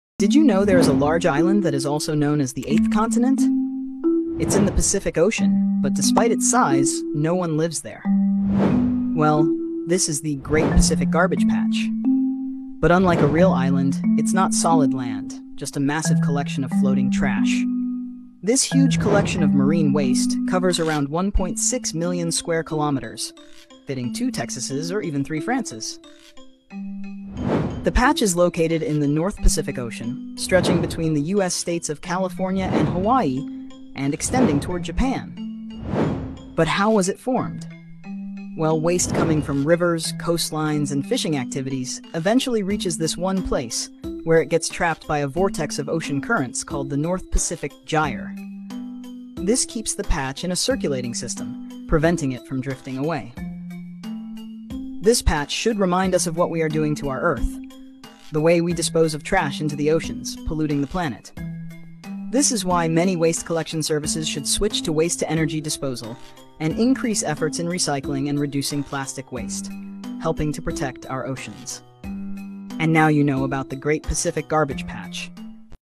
The Island Made of Trash？! 🌍♻️
An island made of trash?! 🗑️🏝️ Yes, it's real—and it's growing! Dive into the shocking truth about the Great Pacific Garbage Patch 🌊 and how it's affecting our oceans, marine life 🐠, and even us. Learn why this floating dump site is a global environmental crisis 🌍⚠️ and what we can do to fight it ♻️.